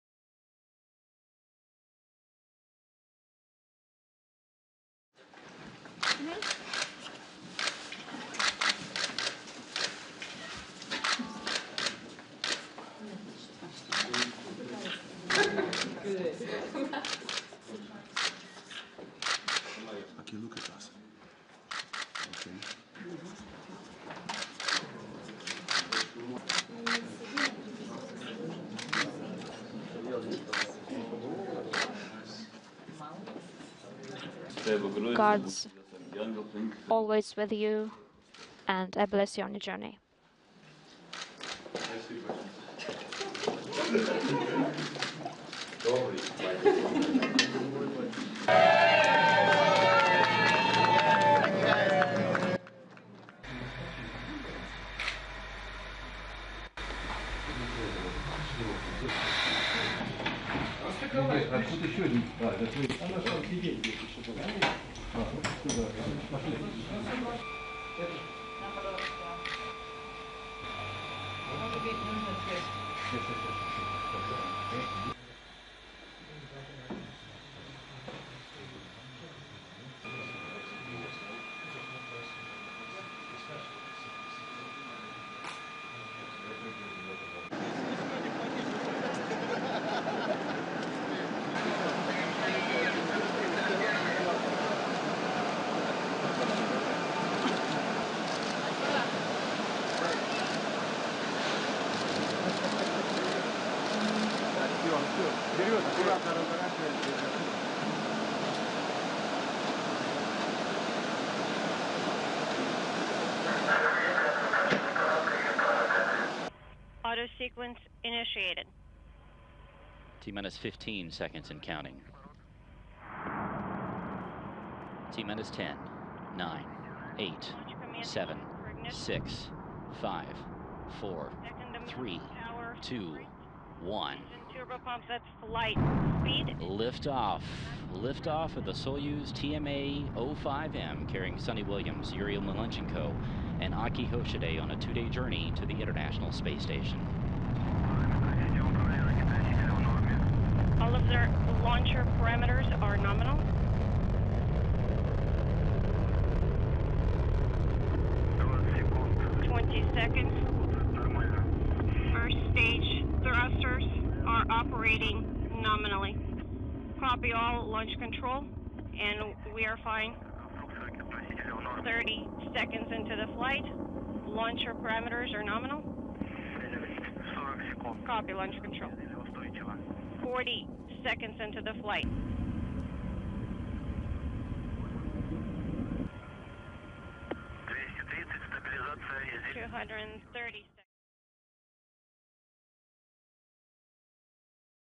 New Expedition 32 Trio Launches to Station
Expedition 32 Flight Engineers Suni Williams, Yuri Malenchenko and Aki Hoshide launched aboard the Soyuz TMA-05M spacecraft at 10:40 p.m. EDT Saturday (8:40 a.m. Kazakhstan time on Sunday) from Baikonur Cosmodrome, Kazakhstan.